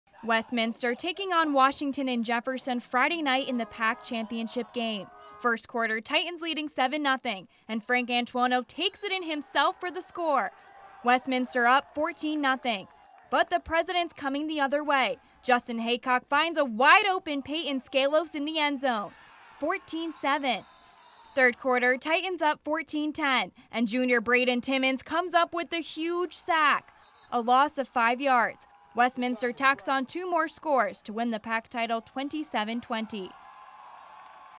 Highlights: Westminster downs Washington and Jefferson to win PAC title
Cole Konieczka threw for 108 yards and two scores